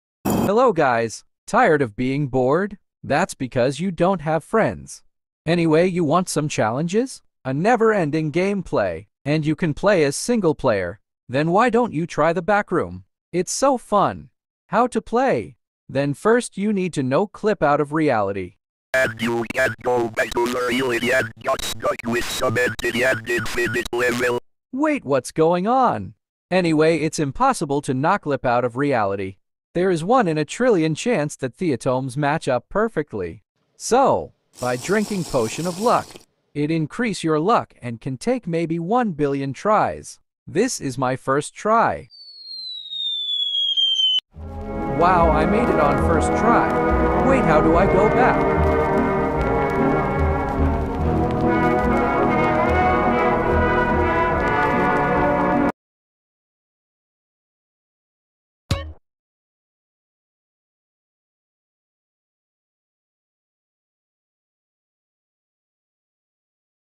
Gladiator Teach How to get on Backroom But in animation (TDS Meme)
Well he just got stuck backrooms or he can get out
i think hack fly it
You know there 2 tower the stuck here scout and spy
Credits to @Oof Warrior

The video Original "Gladiator Teach How to get on Backroom" by "
Oof Warrior"